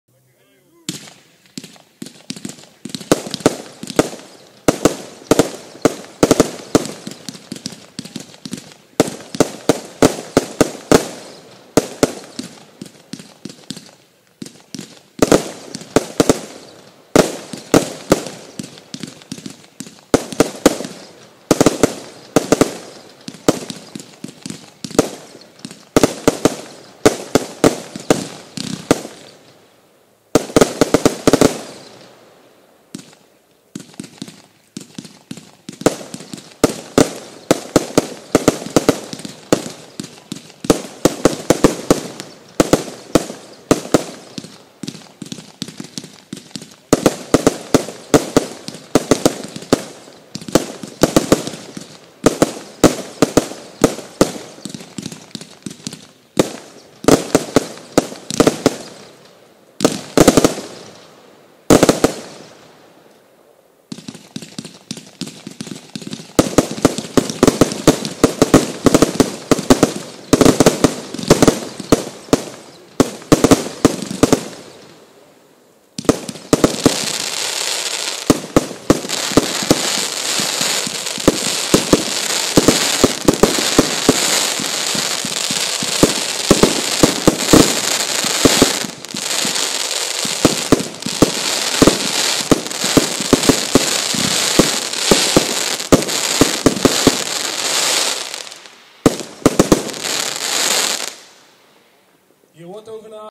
W.T.F. Box - Cafferata Vuurwerk | HD
De W.T.F. Box werd geschoten op de vuurwerk demo van Cafferata Vuurwerk in Uden.